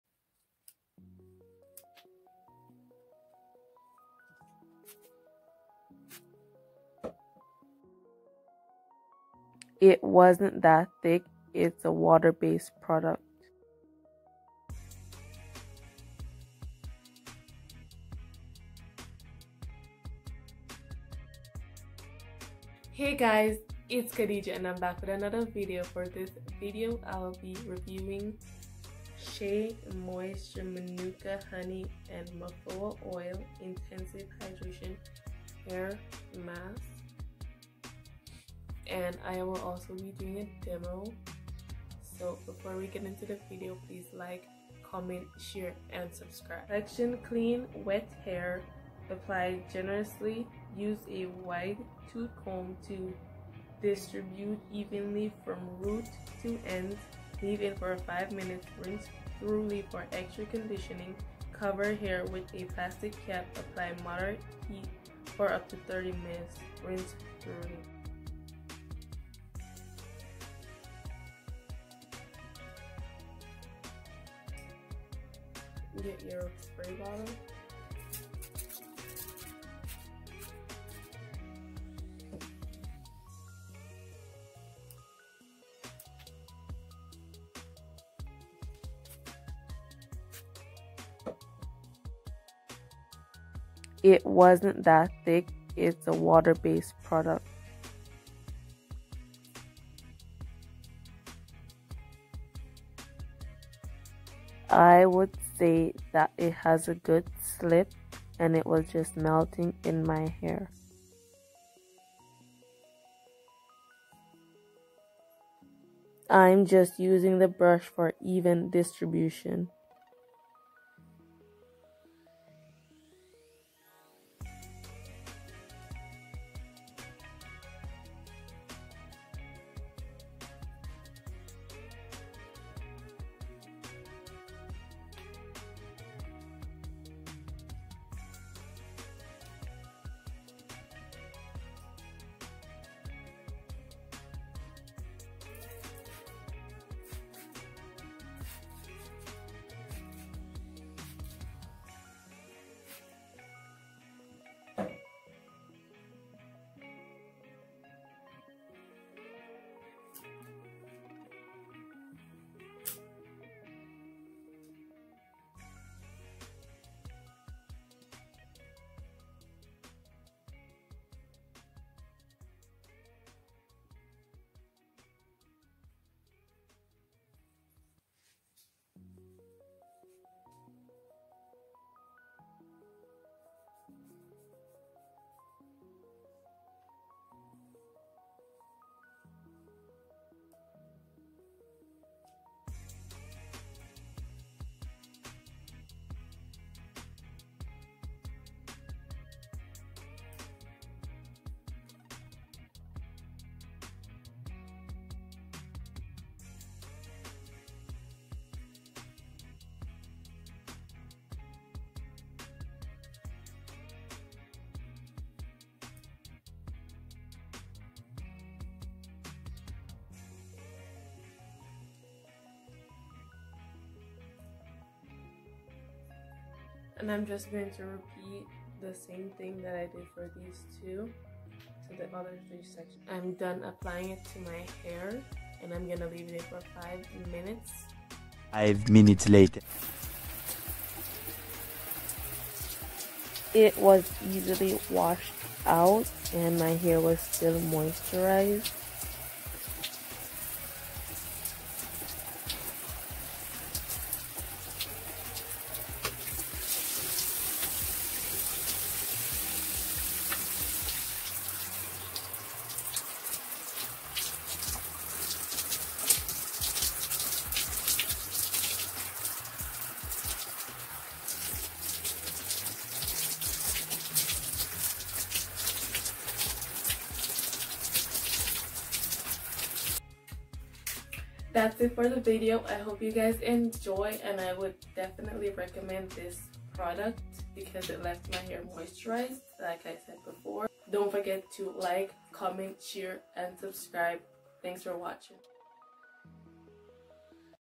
Shea Moisture Manuka Honey & Mafura Oil Intensive Hydration Hair Masque
Hey guys itz kadejah I'm back another video for this video I'll be reviewing + demo with shea moisture Manuka honey and mafura oil intensive hydration hair Masque hope you guys enjoy and don't forget to click the like and Subscribe button.